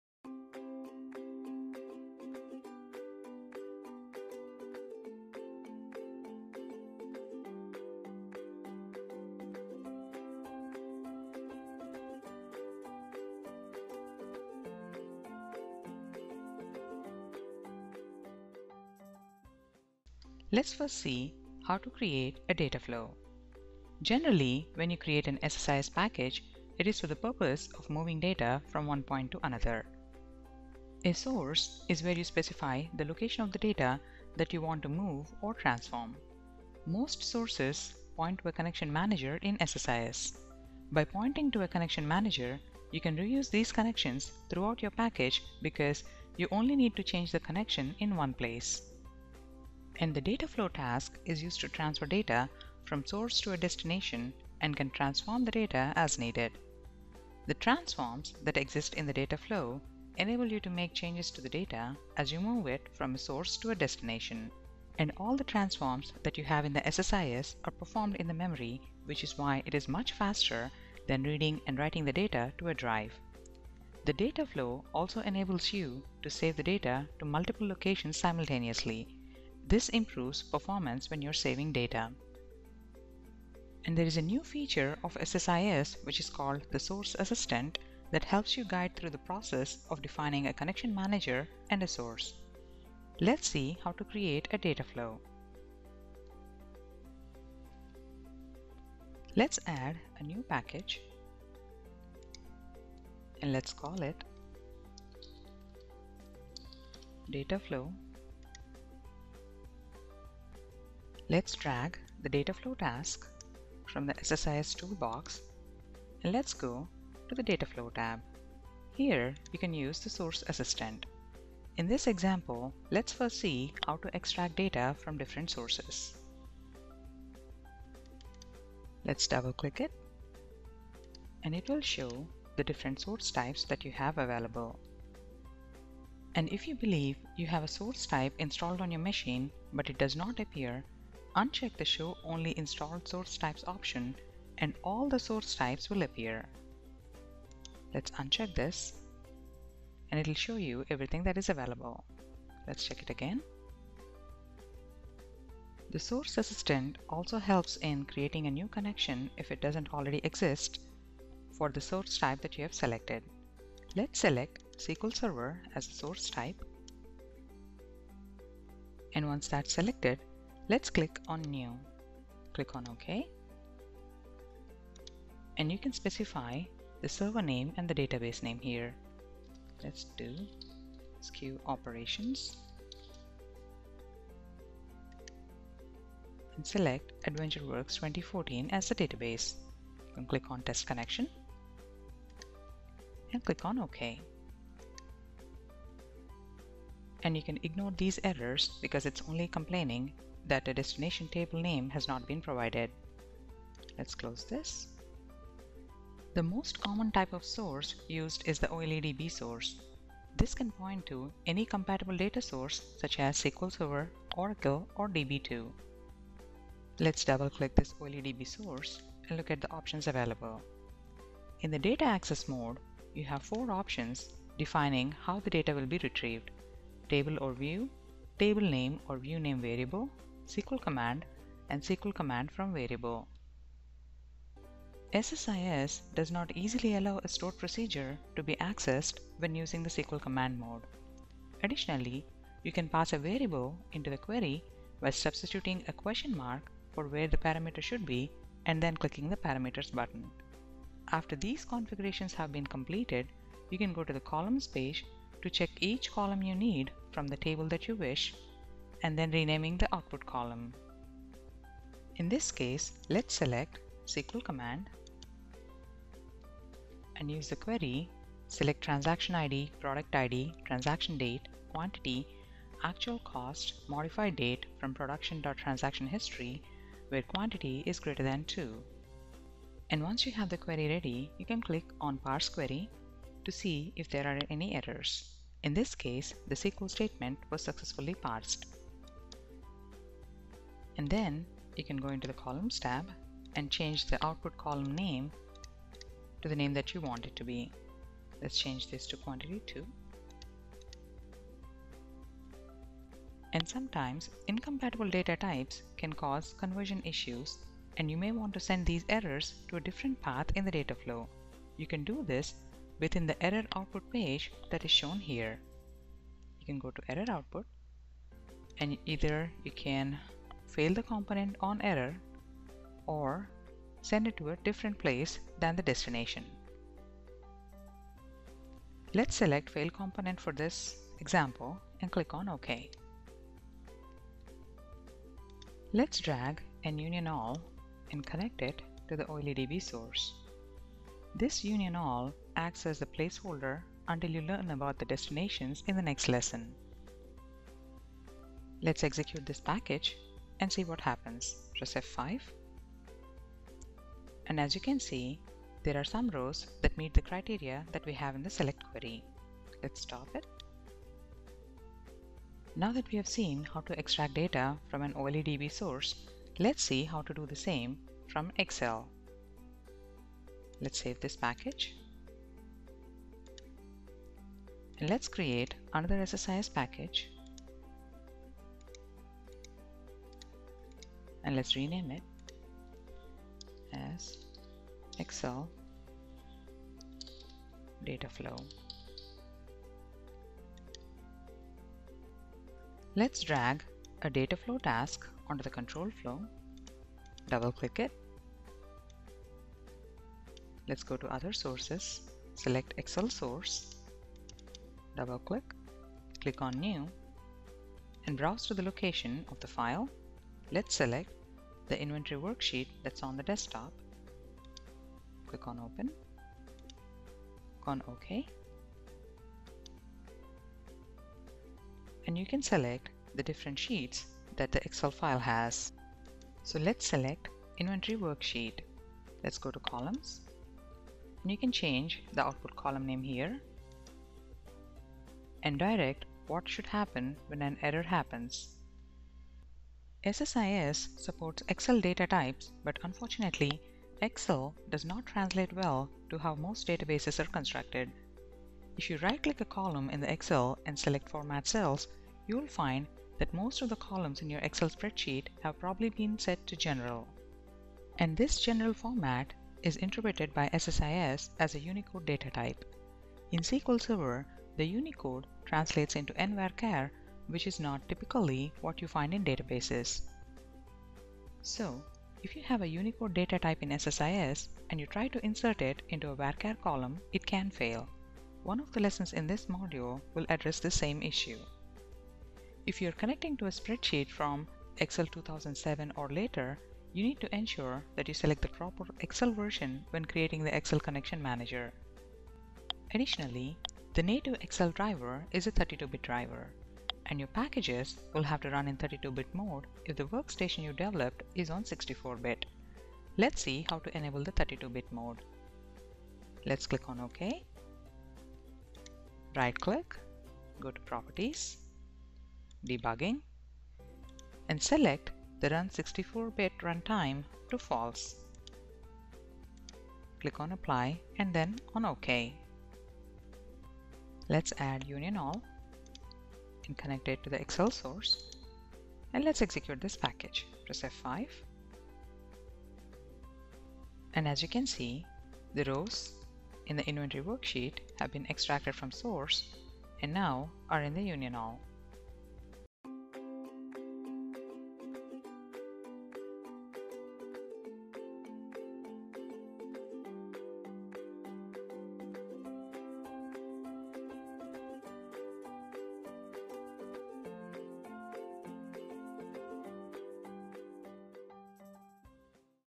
ETL Developer Training - Creating data flow and extracting and loading data
This video talks about how to create a data flow and how to extract and load data using data flow in SSIS (SQL Server 2014).